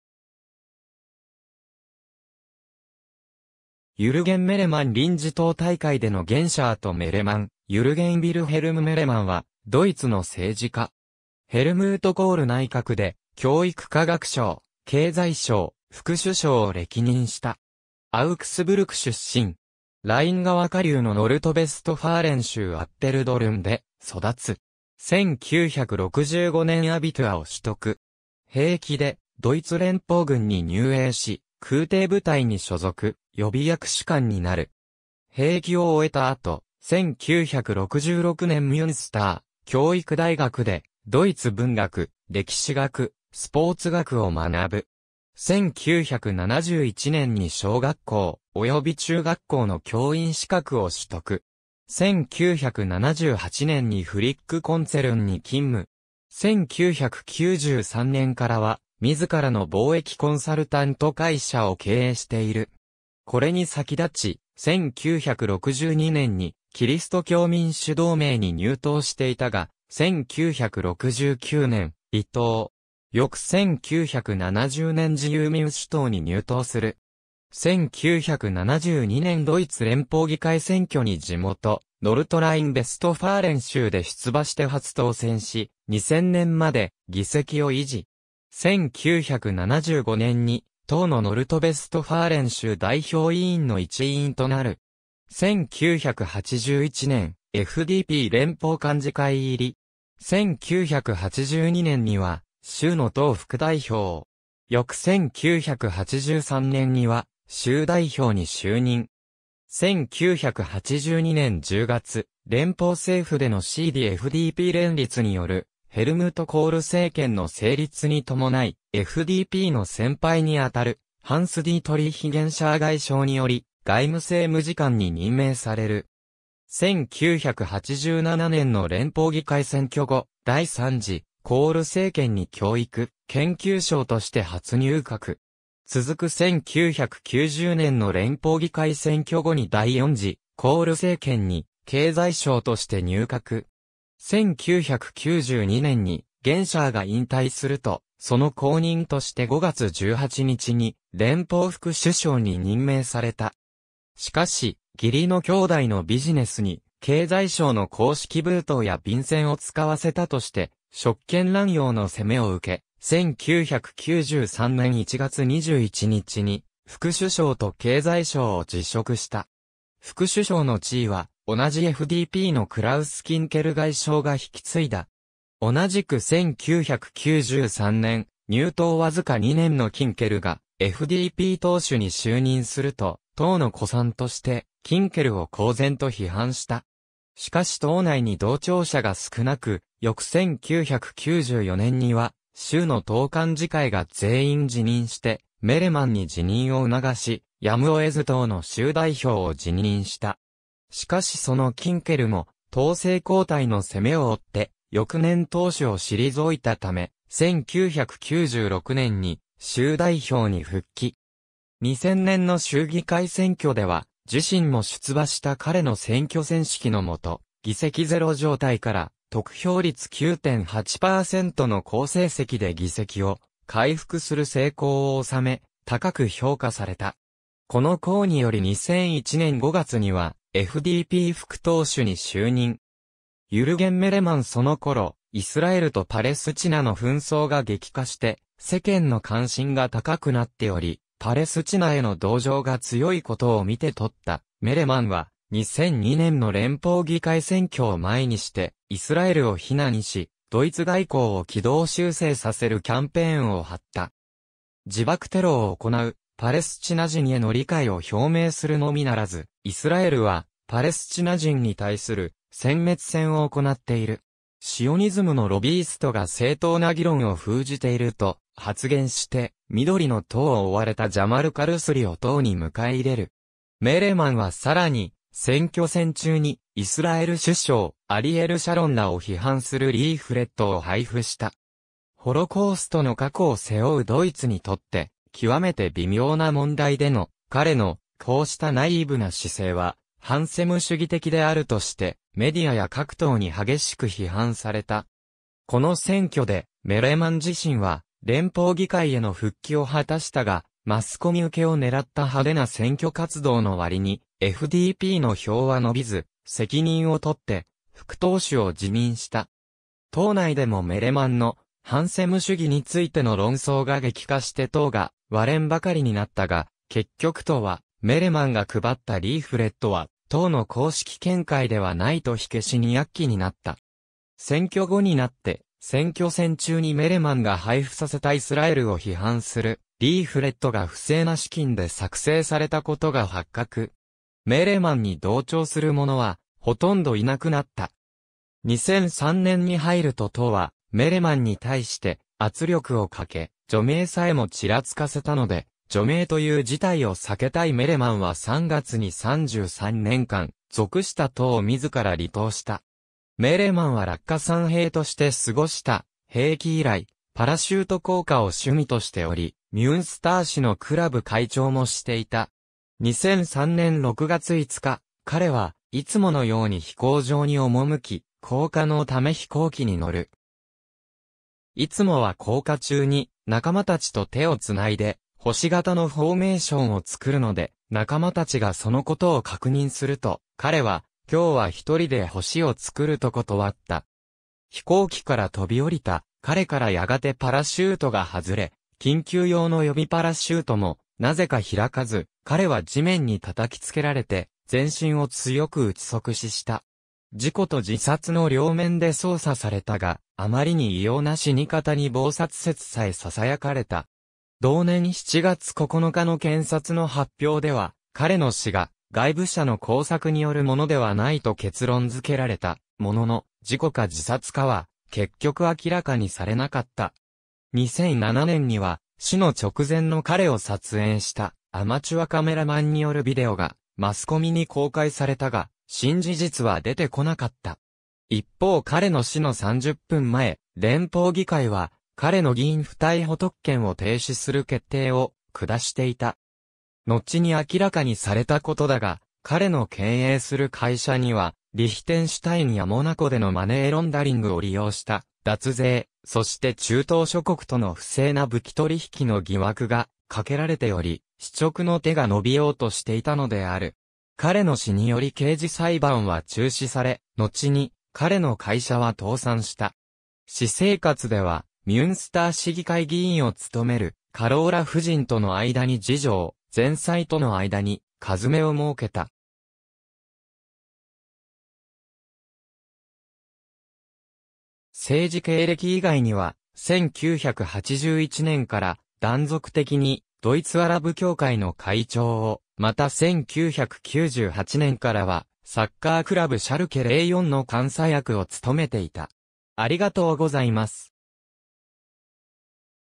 ユルゲン・メレマン
ユルゲン・メレマン（2002年） 臨時党大会でのゲンシャー（左）とメレマン（右）（1980年、フライブルク） ユルゲン・ヴィルヘルム・メレマン（Jürgen Wilhelm Möllemann、1945年7月15日 – 2003年6月5日）は、ドイツ（再統一前は西ドイツ）の政治家。
ヘルムート・コール内閣で教育科学相、経済相、副首相を歴任した。
アウクスブルク出身。
ライン河下流のノルトライン＝ヴェストファーレン州アッペルドルン（現カルカー市と合併）で育つ。
1965年アビトゥア（大学入学資格）を取得。
兵役でドイツ連邦軍に入営し、空挺部隊に所属、予備役士官になる。
兵役を終えた後、1966年ミュンスター教育大学でドイツ文学、歴史学、スポーツ学を学ぶ。
1971年に小学校及び中学校の教員資格を取得。
1978年にフリック・コンツェルンに勤務。
1993年からは自らの貿易コンサルタント会社を経営している。
これに先立ち、1962年にキリスト教民主同盟（CDU）に入党していたが、1969年離党。
翌1970年自由民主党（FDP）に入党する。
1972年ドイツ連邦議会選挙に地元ノルトライン・ヴェストファーレン州で出馬して初当選し、2000年まで議席を維持。
1975年に党のノルトライン＝ヴェストファーレン州代表委員の一員となる。
1981年、FDP連邦幹事会入り。
1982年には州の党副代表。
翌1983年には州代表に就任。
1982年10月、連邦政府でのCDU・FDP連立によるヘルムート・コール政権の成立に伴い、FDPの先輩にあたるハンス・ディートリヒ・ゲンシャー外相により外務政務次官に任命される。
1987年の連邦議会選挙後、第3次コール政権に教育・研究相として初入閣。
続く1990年の連邦議会選挙後に第4次コール政権に経済相として入閣。
1992年にゲンシャーが引退すると、その後任として5月18日に連邦副首相に任命された。
しかし義理の兄弟のビジネスに経済省の公式封筒や便箋を使わせたとして職権濫用の責めを受け、1993年1月21日に副首相と経済相を辞職した。
副首相の地位は同じFDPのクラウス・キンケル外相が引き継いだ。
同じく1993年、入党わずか2年のキンケルがFDP党首に就任すると、党の古参としてキンケルを公然と批判した。
しかし党内に同調者が少なく、翌1994年には州の党幹事会が全員辞任してメレマンに辞任を促し、やむを得ず党の州代表を辞任した。
しかしそのキンケルも党勢後退の責めを負って翌年党首を退いたため、1996年に州代表に復帰。
2000年の州議会選挙では、自身も出馬した彼の選挙戦指揮の下、議席ゼロ状態から得票率9.8％の好成績で議席を回復する成功を収め、高く評価された。
この功により2001年5月にはFDP副党首に就任。
ユルゲン・メレマン（2002年の総選挙時） その頃イスラエルとパレスチナの紛争が激化して世間の関心が高くなっており、パレスチナへの同情が強いことを見てとったメレマンは、2002年の連邦議会選挙を前にして、イスラエルを非難しドイツ外交を軌道修正させるキャンペーンを張った。
自爆テロを行うパレスチナ人への理解を表明するのみならず、「イスラエルはパレスチナ人に対する殲滅戦を行っている。シオニズムのロビイストが正当な議論を封じている」と発言して緑の党を追われたジャマル・カルスリを党に迎え入れる。
メレマンはさらに、選挙戦中にイスラエル首相アリエル・シャロンらを批判するリーフレットを配布した。
ホロコーストの過去を背負うドイツにとってきわめて微妙な問題での、彼のこうしたナイーブな姿勢は、反セム主義的であるとしてメディアや各党に激しく批判された。
この選挙でメレマン自身は連邦議会への復帰を果たしたが、マスコミ受けを狙った派手な選挙活動の割にFDPの票は伸びず、責任を取って副党首を辞任した。
党内でもメレマンの「反セム主義」についての論争が激化して党が割れんばかりになったが、結局党は「メレマンが配ったリーフレットは党の公式見解ではない」と火消しに躍起になった。
選挙後になって、選挙戦中にメレマンが配布させたイスラエルを批判するリーフレットが不正な資金で作成されたことが発覚。
メレマンに同調する者はほとんどいなくなった。
2003年に入ると党はメレマンに対して圧力をかけ除名さえもちらつかせたので、除名という事態を避けたいメレマンは3月に33年間属した...